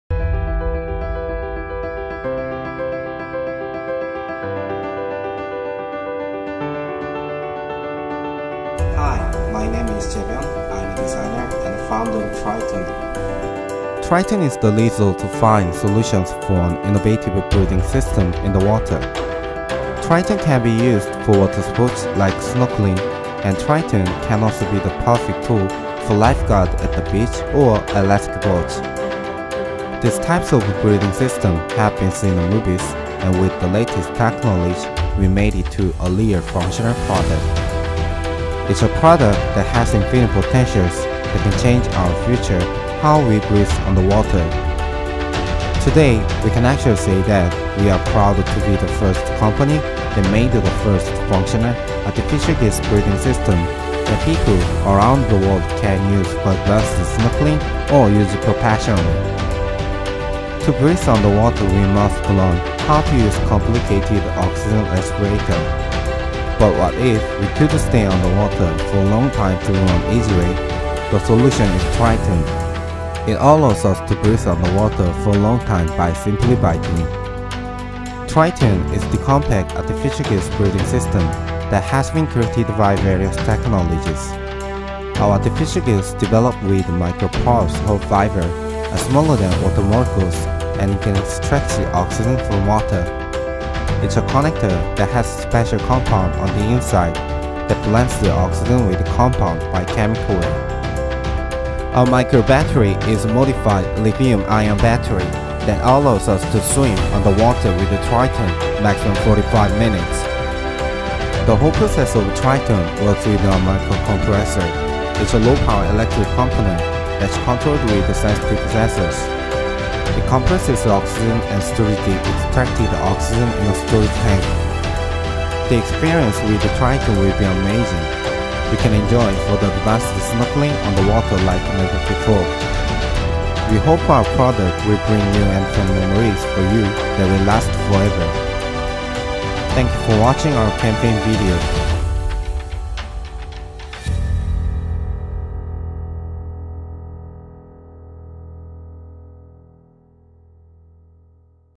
Triton : La branchie artificielle qui permet de respirer sous l'eau
Le designer coréen Jeabyun Yeon présente son invention appelée "Triton" qui permet de respirer sous l'eau pendant 45 minutes. Le concept se rapproche d'une branchie artificielle qui extrait l'oxygène de l'eau grâce à une fibre microporeuse constituée de minuscules trous. Un microcompresseur alimenté par une microbatterie permet de stocker l'oxygène et de le distribuer au fur et à mesure à l'utilisateur. En revanche, la profondeur maximum d'utilisation est de 5 mètres. Le designer et son équipe ont lancé un financement participatif via Indiegogo qui a permis de récolter 680 000 euros en 6 jours, soit 15 fois plus que la somme nécessaire de 50 000$ demandée. Une invention qui rappelle le gadget qu'utilisent Obi Wan Kenobi et Qui Gon Ginn dans "Star Wars : La menace fantôme" ou encore James Bond dans "Opération Tonnerre". Le Triton est en précommande à partir de 150 dollars, soit un peu plus de 130 euros.